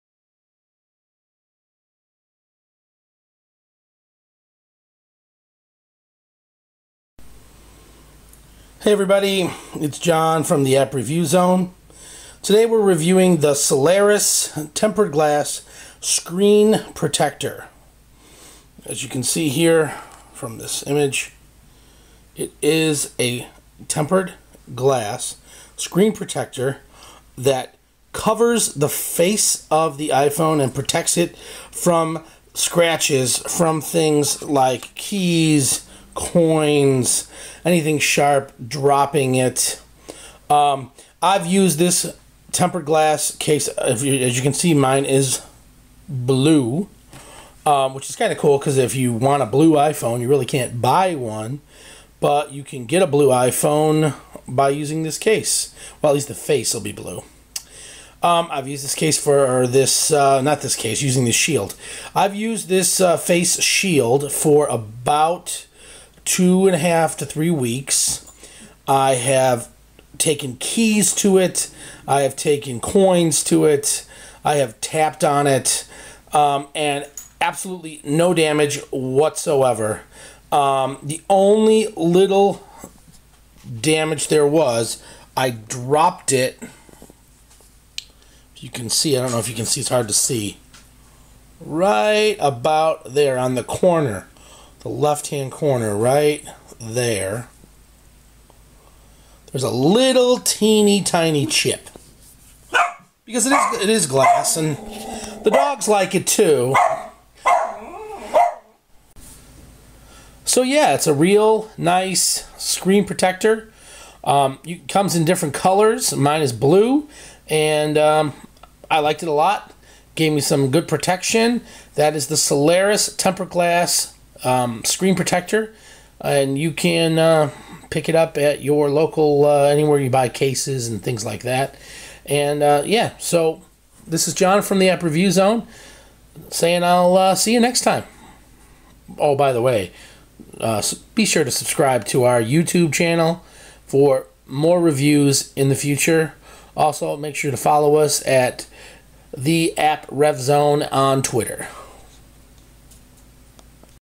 Cellairis Tempered Glass
Cellairis Tempered Glass Video Product Review - This video talks about all the benefits and reliability of the Cellars Tempered Glass screen protector

My Video Schedule

Friday - New Reviews Posted